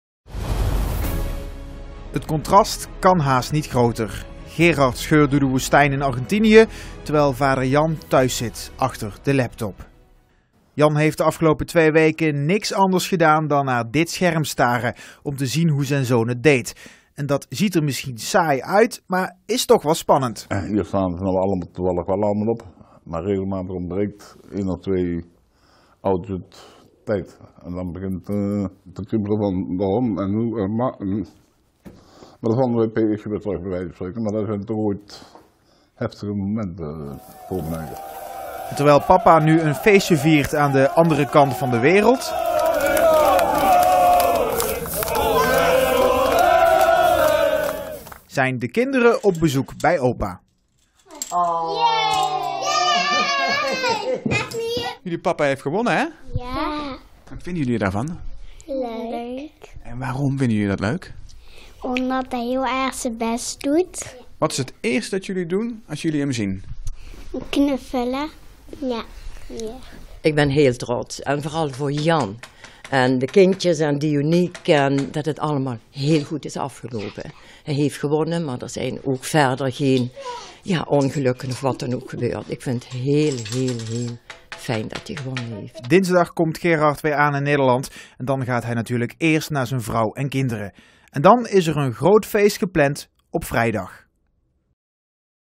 Jan de Rooy is beretrots op zijn zoon Gerard de Rooy
Jan de Rooy is vol lof over de prestatie van z’n zoon Gerard in de Dakar Rally. Gerard de Rooy won de rally voor de tweede keer. “Hij heeft geen enkele fout gemaakt”, concludeert zijn vader.